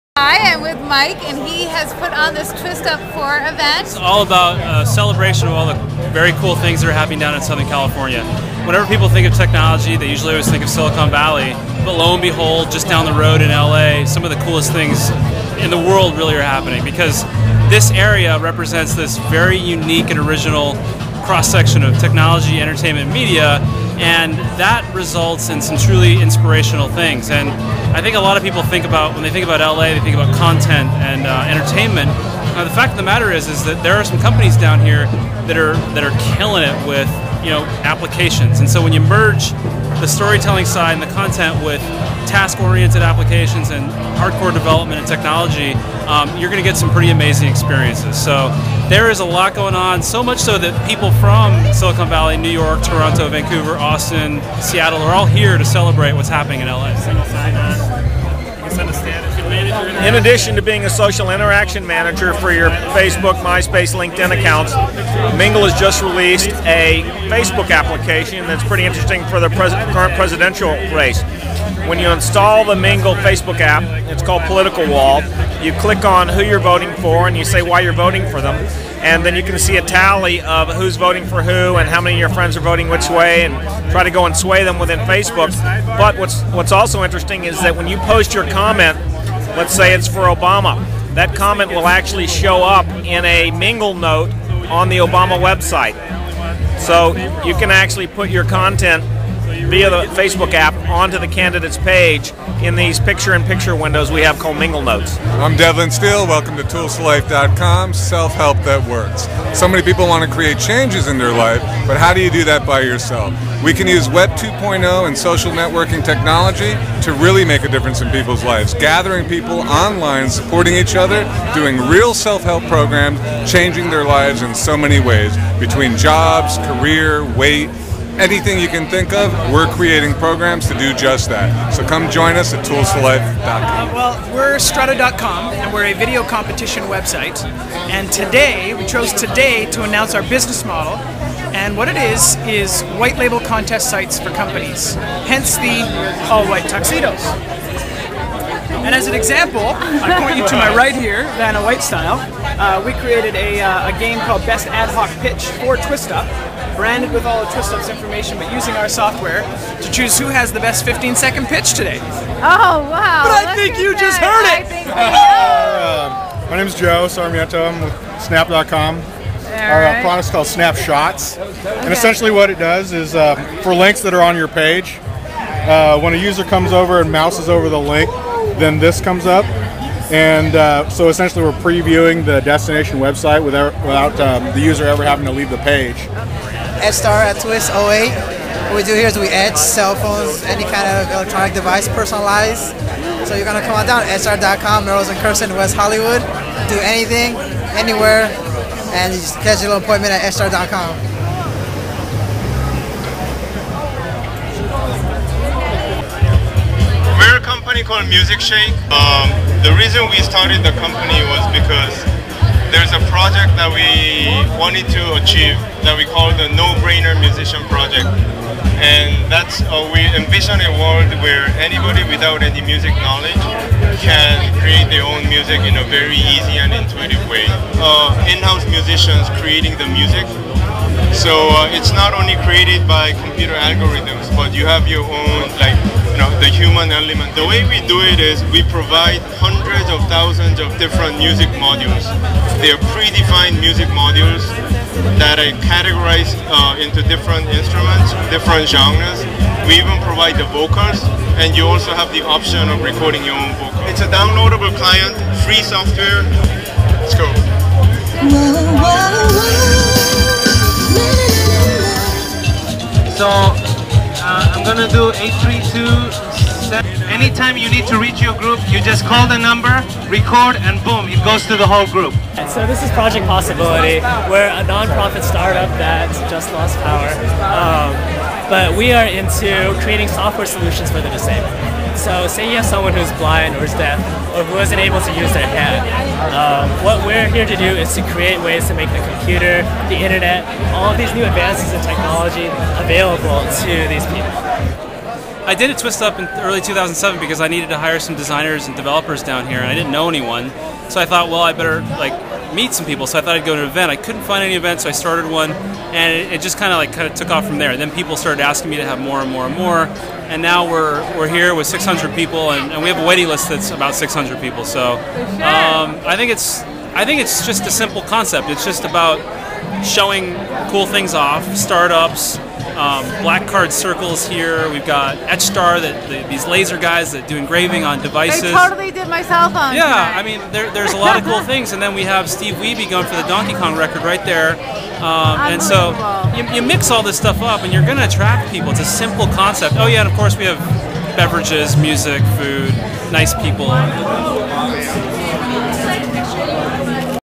Twiistup 4
Tech event honoring inventive start-ups in Los Angeles